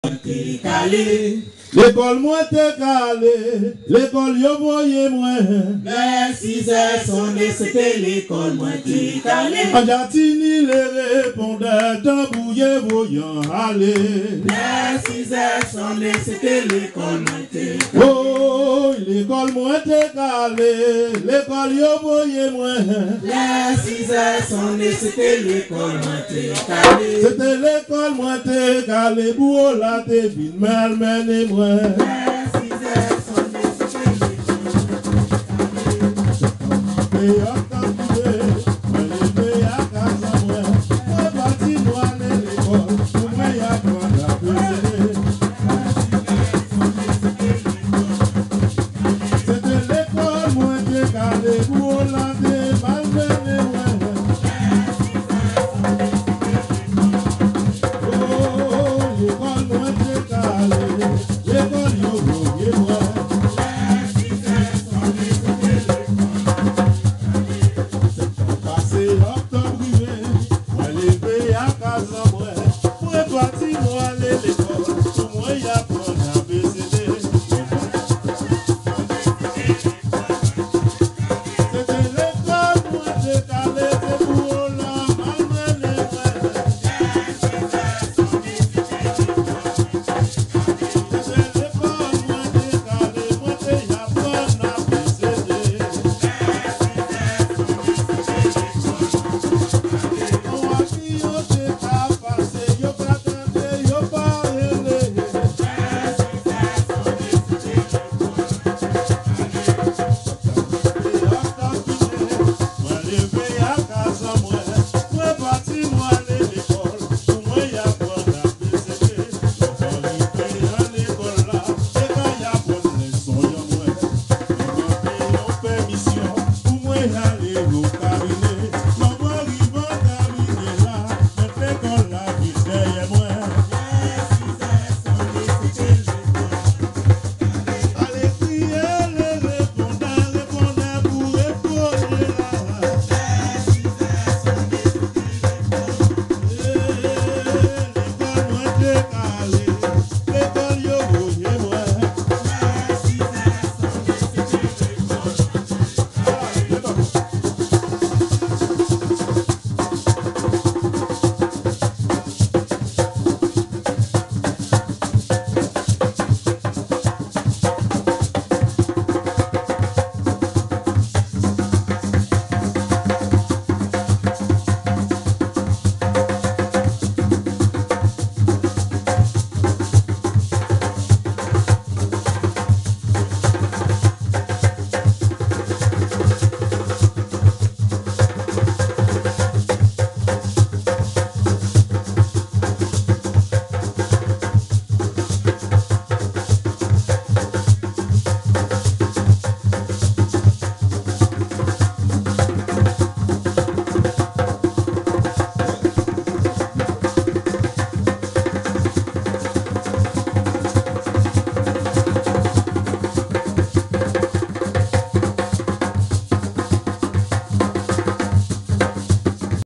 Woulo ,Bwavo ba lé Tanbouyé .Ayayayyyy, Marius , chantè an mwen....GWO KA sé TANBOU! !!
Mizik "GWO KA" , KILTI é TRADISYON dè sa ki tan nou !!!